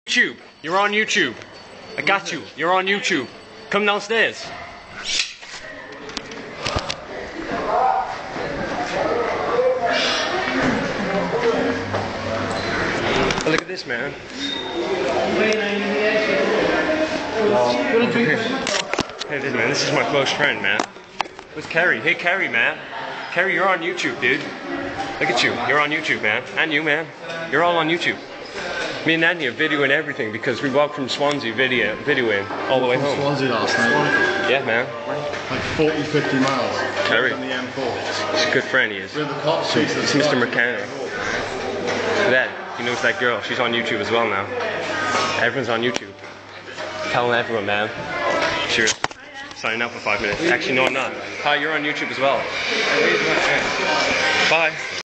Ant and cardiffs road to nowhere
going round college filming people and seeing how they react to being on youtube lol